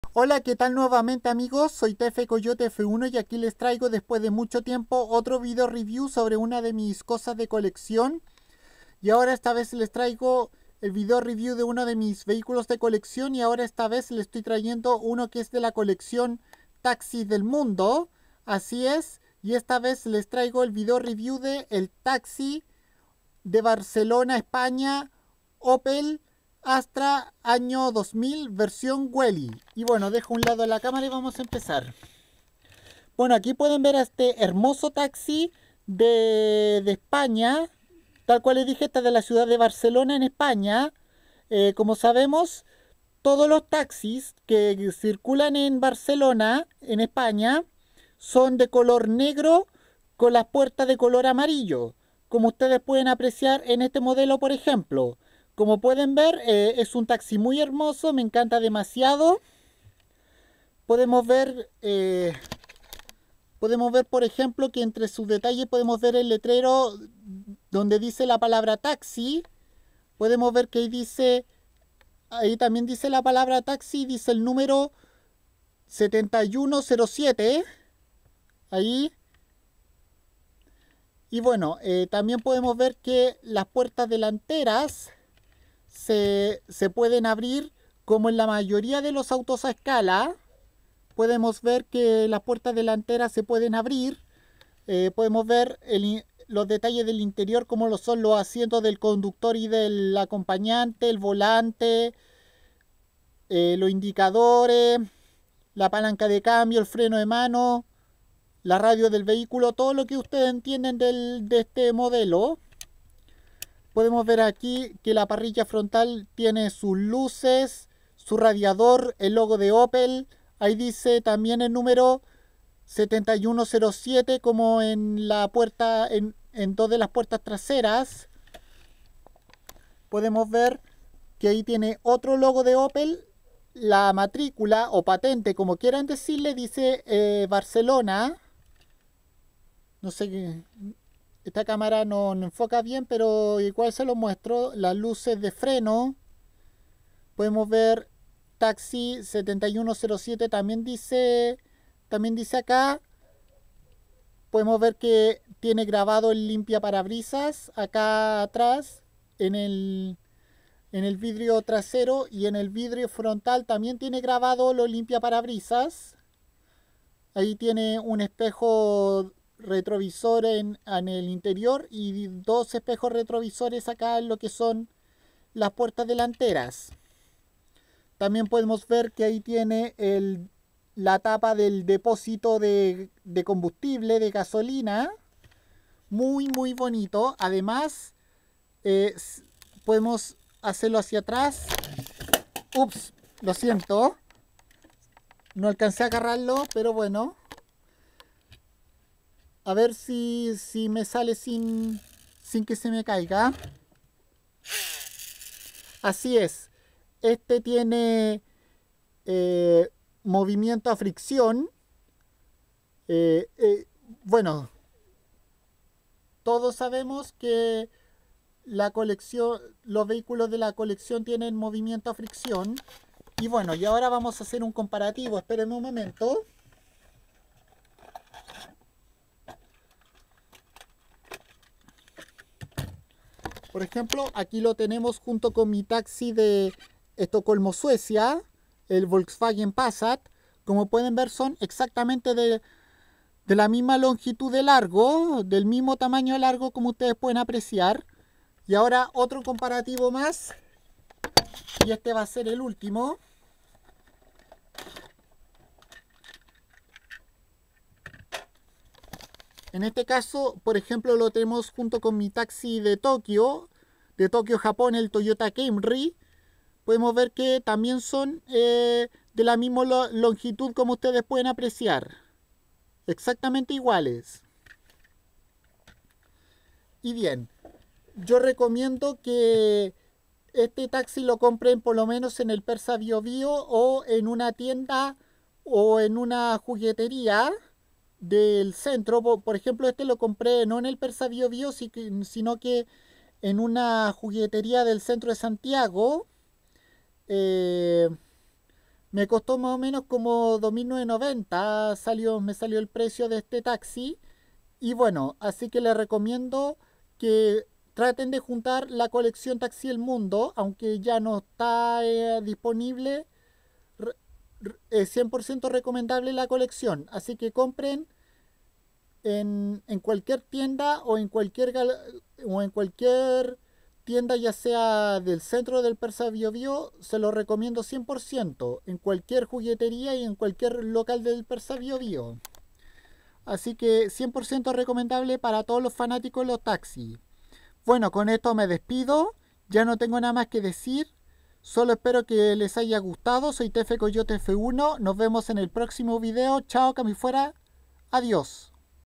Video Review en español Opel Astra 2000 Taxi de Barcelona(España) versión Welly
Parte de la colección Taxis del Mundo y esta vez les traigo un Opel Astra año 2000 que es un taxi de Barcelona España

Después de mucho tiempo reviso una de mis colecciones

Todo comentario ofensivo será inmediatamente borrado